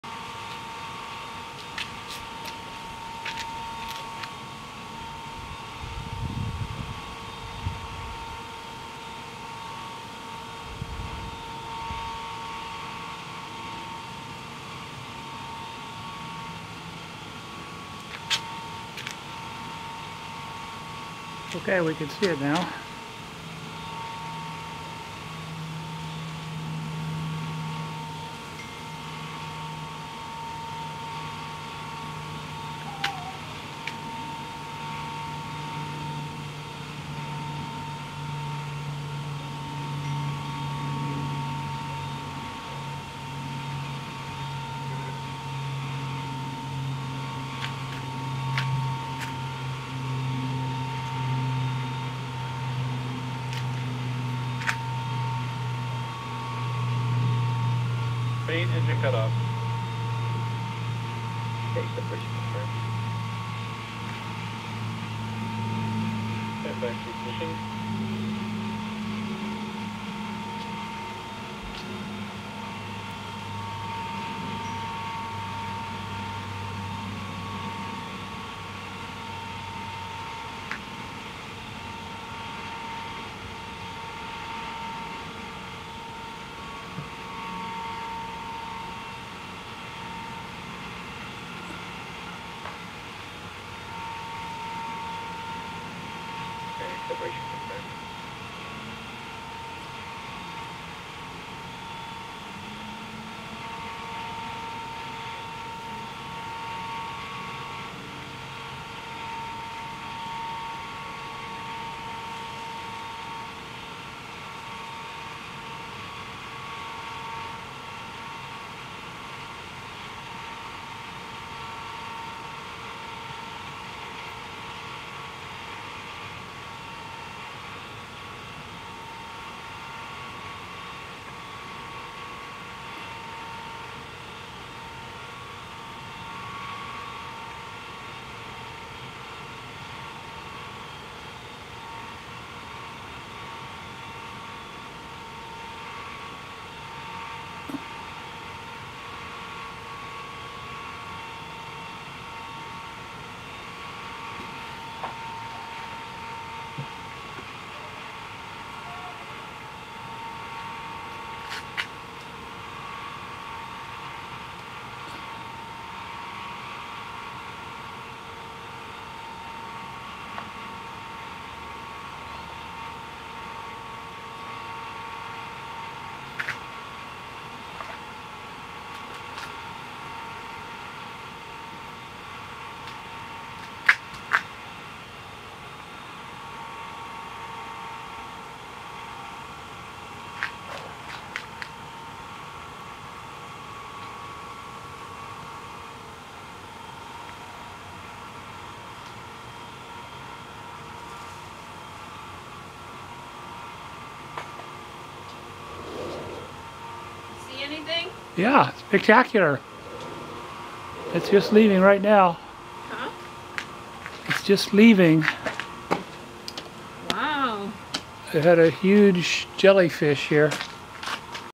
Spacex launch june 18 2024 from Vandenberg
as seen from San Marcos, Ca. Northern San Diego County